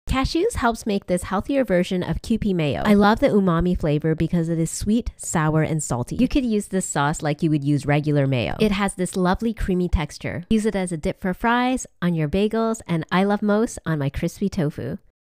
Vegan Kewpie Mayo
I love making my own creamy sauces better than buying mayo. I love this vegan Kewpie mayo. I dip my veggies, fries, breads in this! I try and have this on hand as much as I can 🥰
1 cup cashews
1/2 cup water
3 TBS red wine vinegar
2 TBS maple syrup
1tsp salt
1tsp mustard
1/3 tsp garlic powder